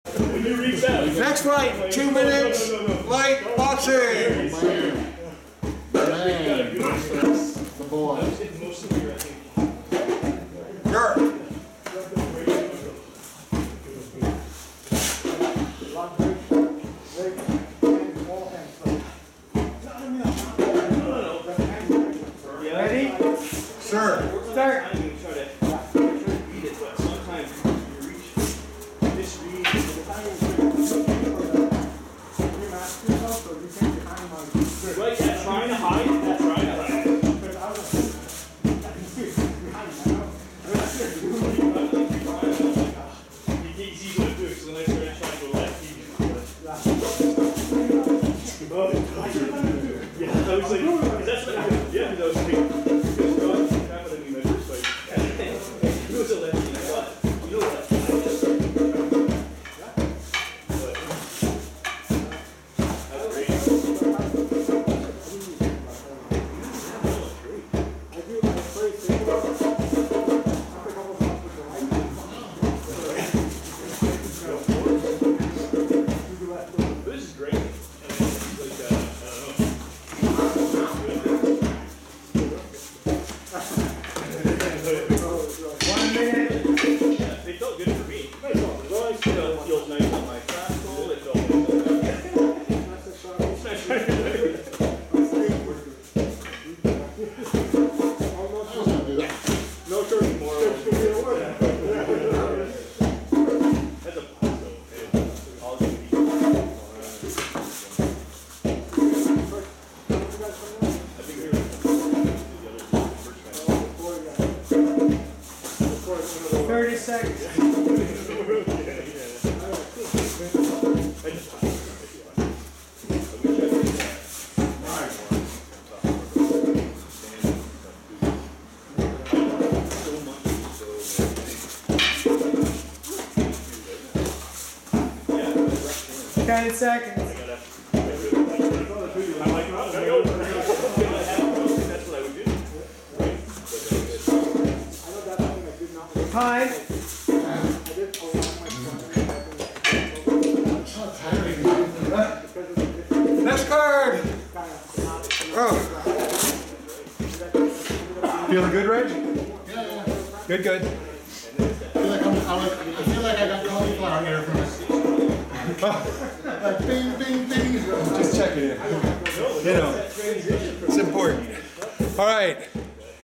00070
Toronto Throwdown Group - This event was run in May 2023. All the fights in this listing are meant for personal review by the fighters. If your comments on any of these videos are anything less than constructive they will be removed.

This is a friendly Sparring and Training event. There are no winners or loser, just fighters learning and developing their art by testing and reviewing their fighting skills against multiple different styles and methods.